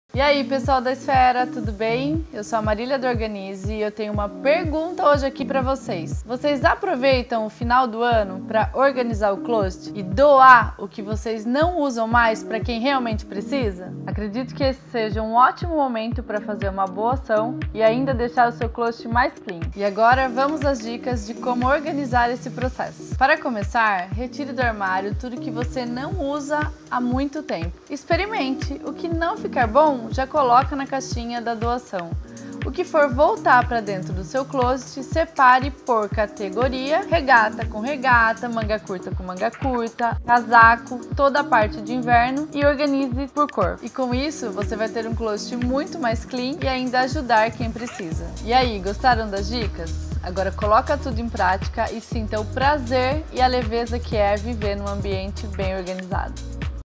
Como organizar o closet no fim de ano - Com Marília Malagueta
Que tal aproveitar o fim do ano para rever o seu closet?

No CadaUmNaSuaEsfera de hoje, a Marília Malagueta da Organnize vai explicar de forma rápida e prática como é possível deixar seu espaço bem mais organizado! ✔️

Lembre-se: se for renovar seu guarda roupas ou precisar de itens de organização passa no Shopping Esfera para ganhar cashback em todas as suas compras! 😍